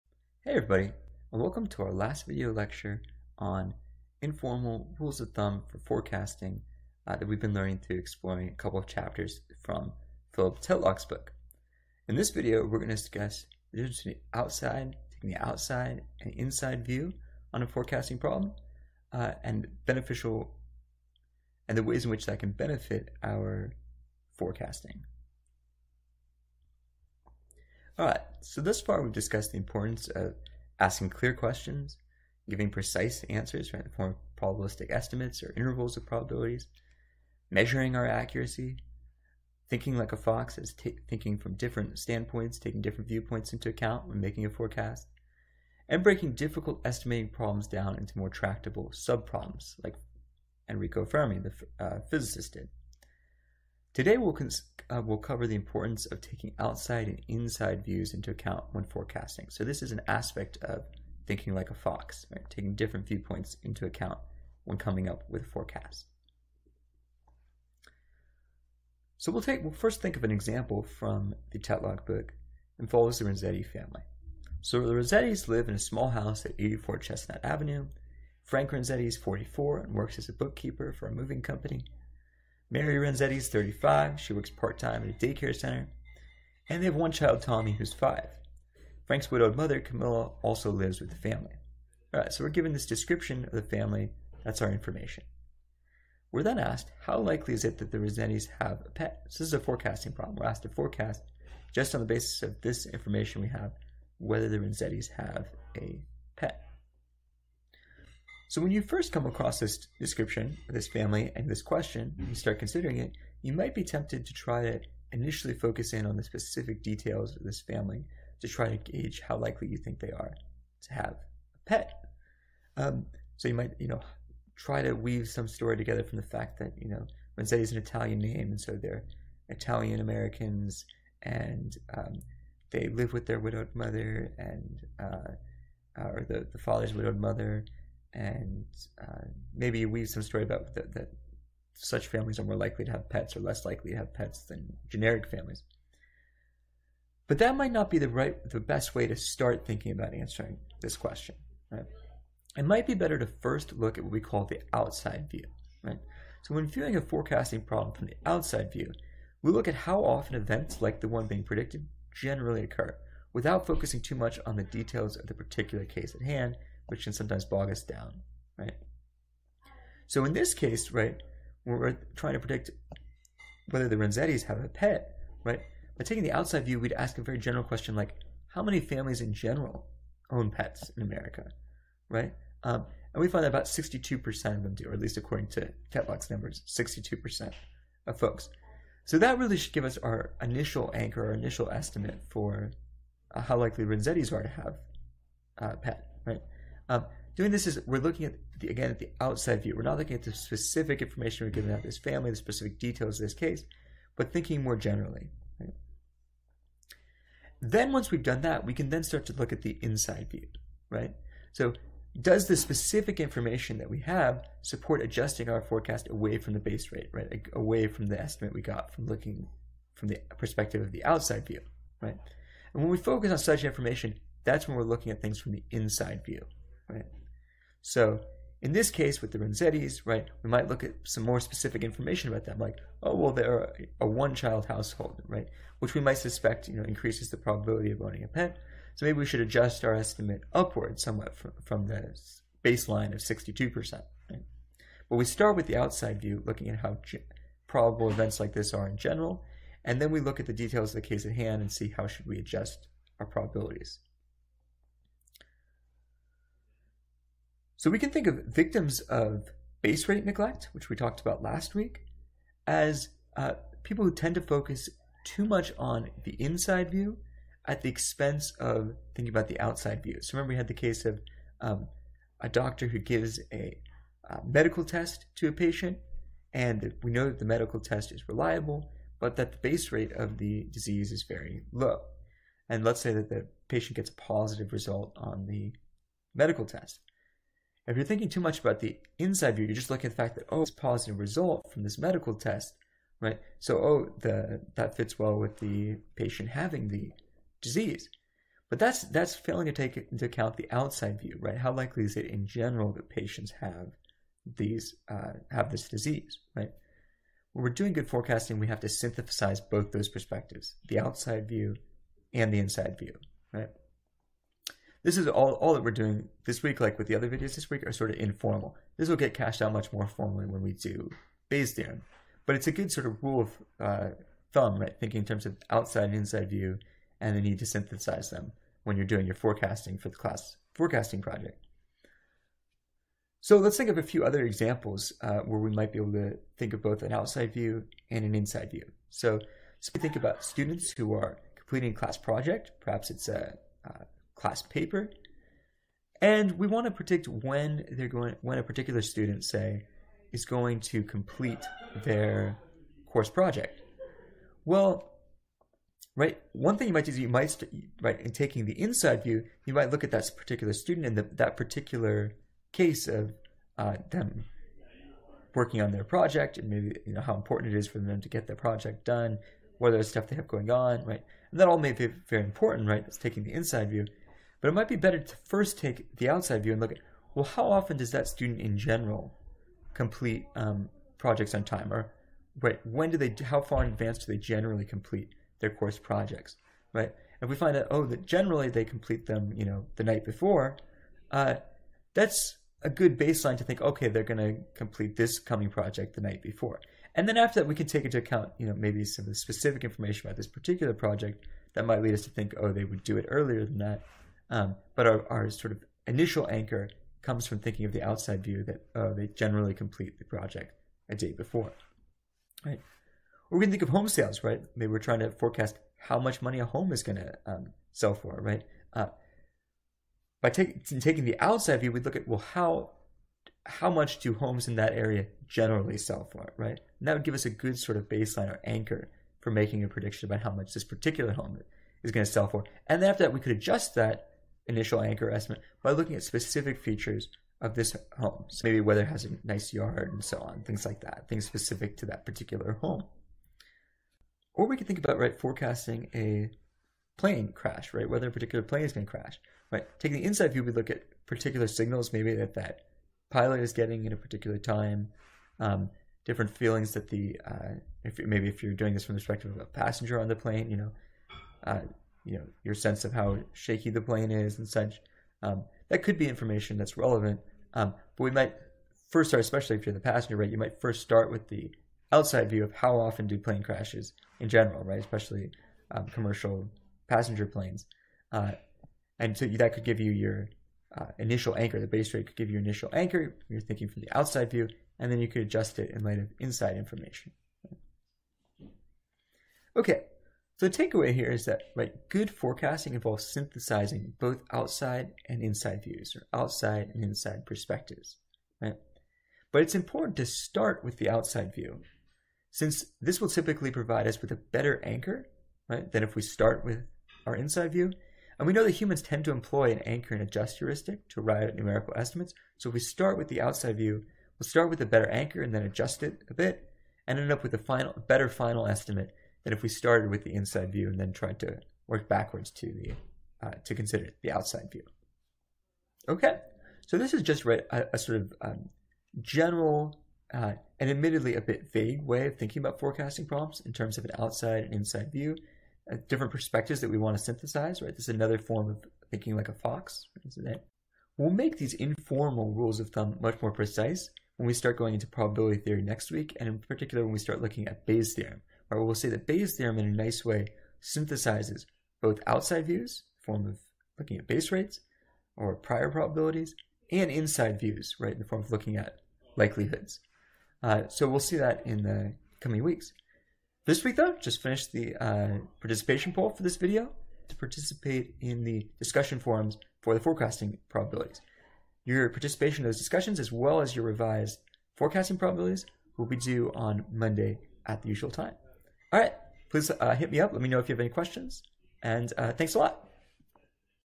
Outside and Inside Views
This video covers the importance of viewing forecasting questions from both Outside and Inside perspectives.